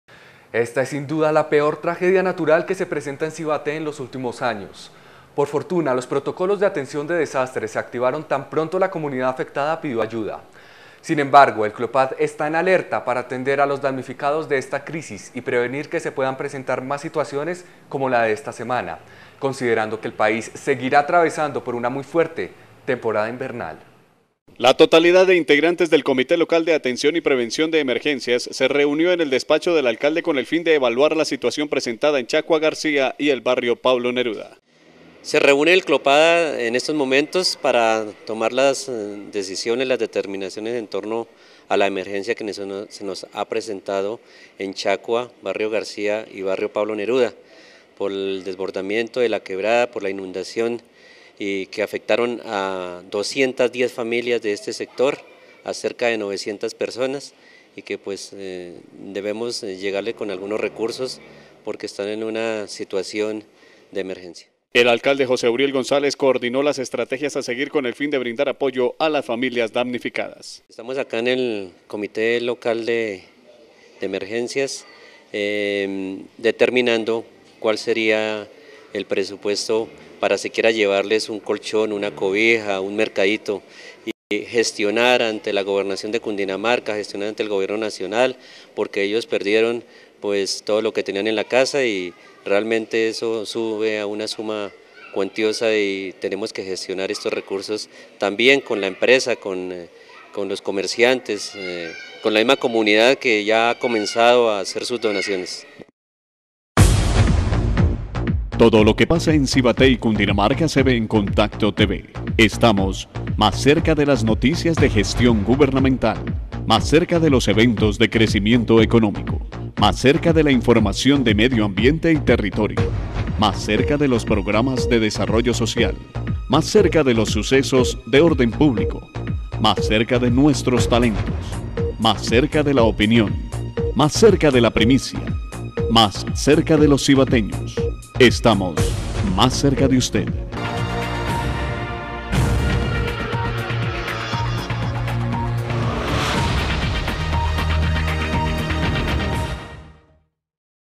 SIBATE COMITE DE EMERGENCIA CLOPAD CTV 42
ESTA ES SIN DUDA LA PEOR TRAGEDIA NATURAL QUE SE PRESENTA EN SIBATÉ EN LOS ÚLTIMOS AÑOS... POR FORTUNA, LOS PROTOCOLOS DE ATENCIÓN DE DESASTRES SE ACTIVARON TAN PRONTO LA COMUNIDAD AFECTADA PIDIÓ AYUDA... SIN EMBARGO, EL CLOPAD ESTÁ EN ALERTA PARA ATENDER A LOS DAMNIFICADOS DE ESTA CRISIS Y PREVENIR QUE SE PUEDAN PRESENTAR MÁS SITUACIONES COMO LA DE ESTA SEMANA, CONSIDERANDO QUE EL PAÍS SEGUIRÁ ATRAVESANDO POR UNA MUY FUERTE TEMPORADA INVERNAL..